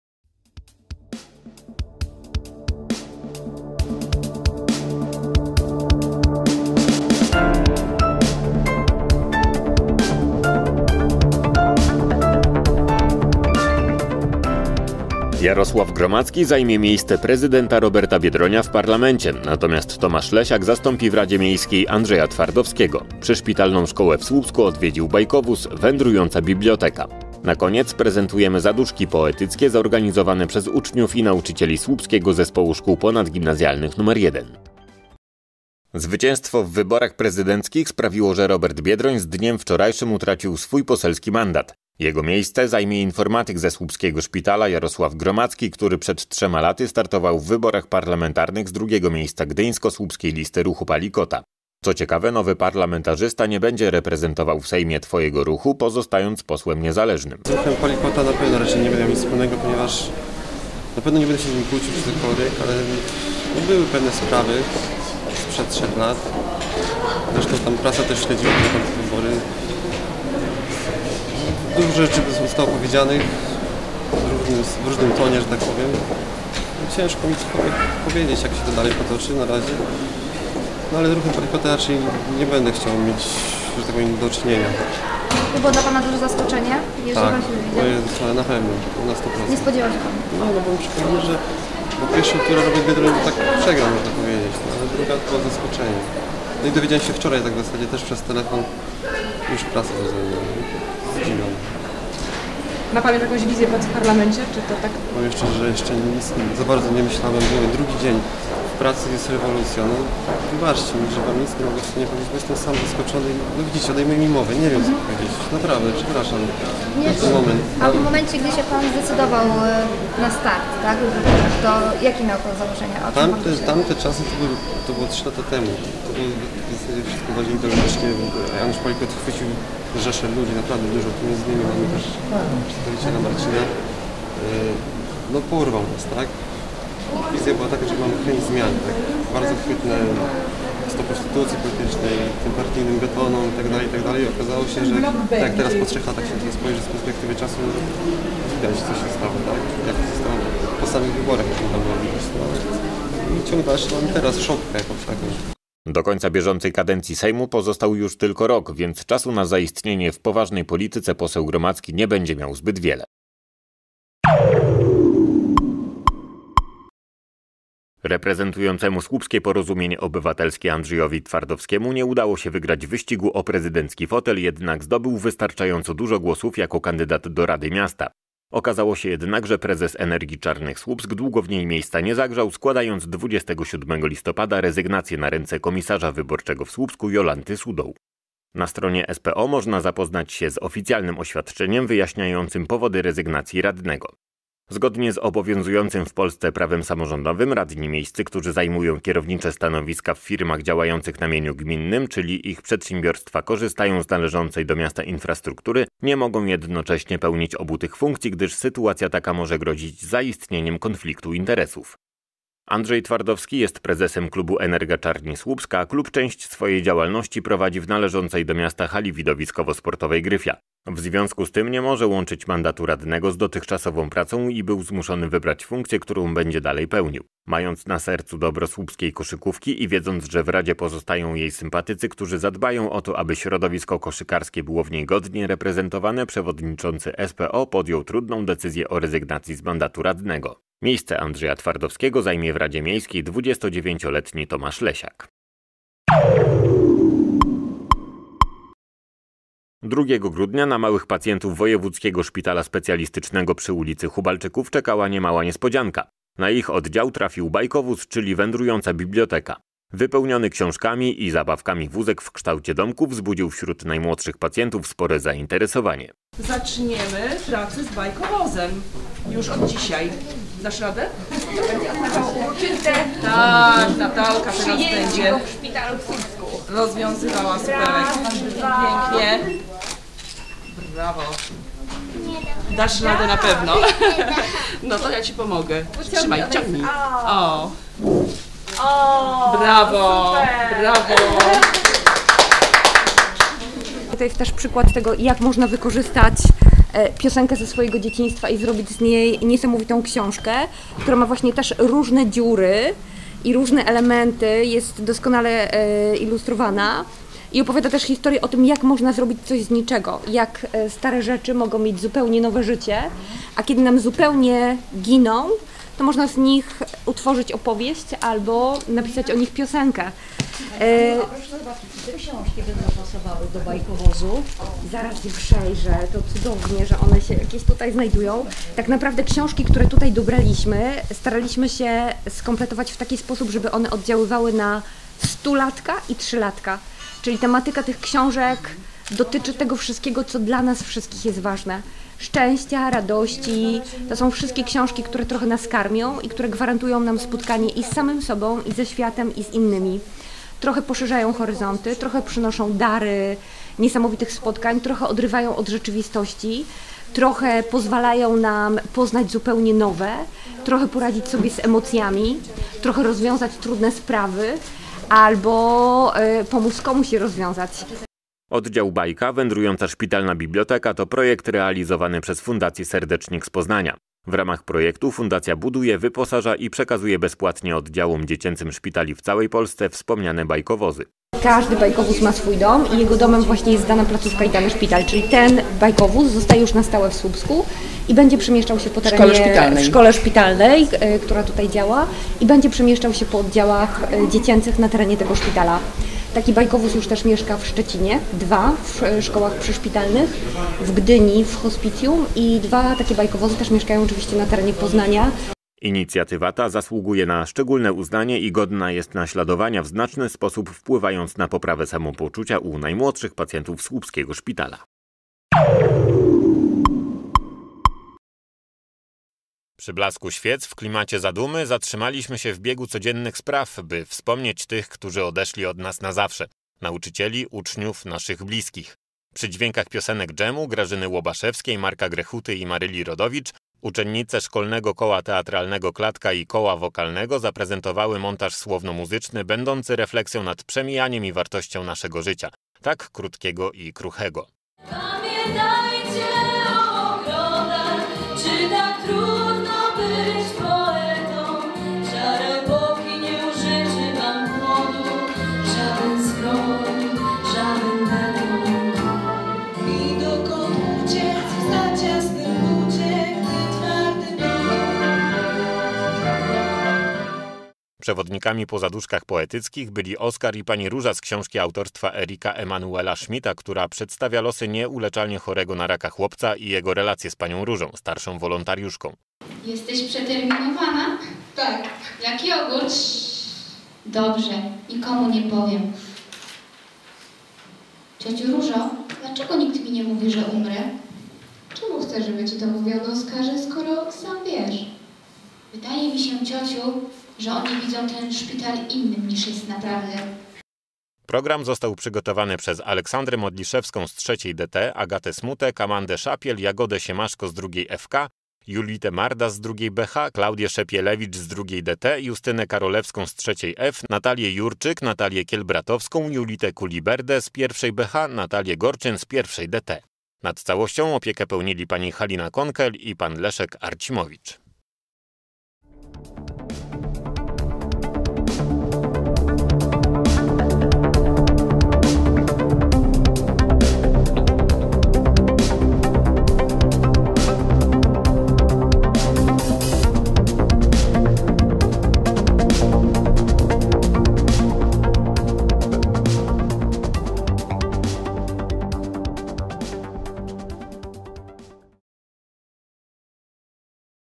Wiadomości Słupsk - 02.12.2014 - 10minut TV
Jarosław Gromadzki zajmie miejsce prezydenta Roberta Biedronia w parlamencie, natomiast Tomasz Lesiak zastąpi w Radzie Miejskiej Andrzeja Twardowskiego. Przyszpitalną szkołę w Słupsku odwiedził Bajkowóz – Wędrująca Biblioteka. Na koniec prezentujemy Zaduszki Poetyckie, zorganizowane przez uczniów i nauczycieli słupskiego Zespołu Szkół Ponadgimnazjalnych nr 1.